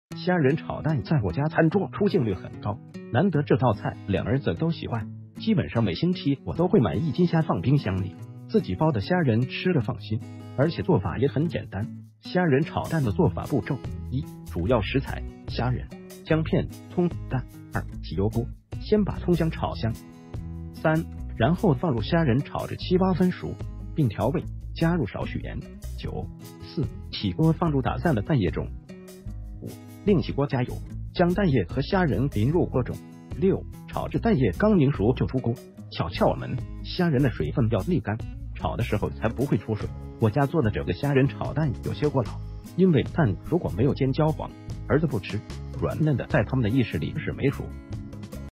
虾仁炒蛋
食材明细
虾仁:适量
蛋:适量
姜片:适量
葱:适量
口味:咸鲜
工艺:炒
耗时:十分钟
难度:简单